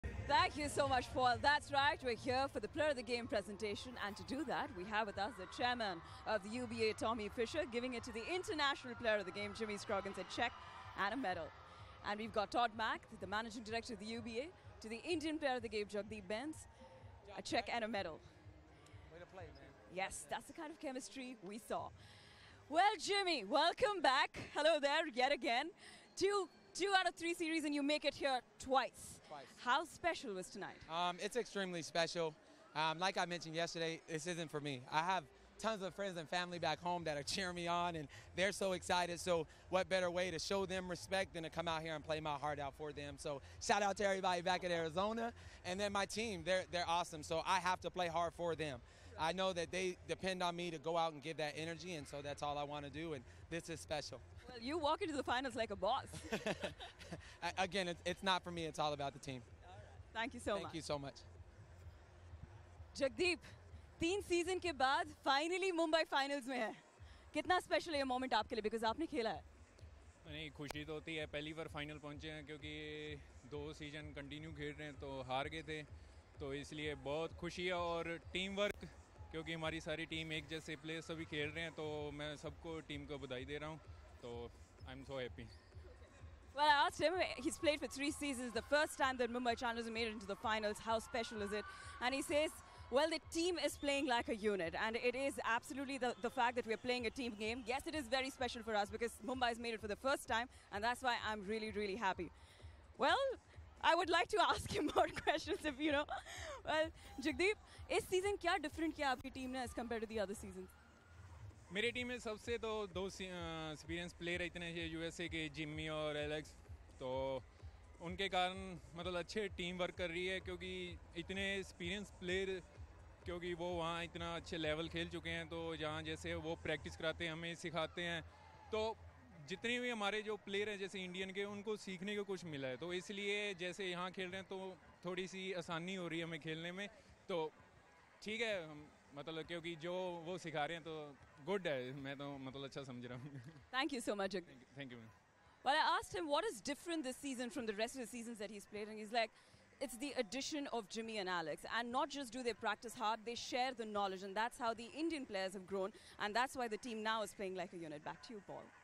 UBA India Playoffs Players of the Game Presentation March 11 Mumbai Challengers
UBA India Playoff Players of the Game Presentation March 11

For highlights, stories and more about UBA Pro Basketball League, head over to our official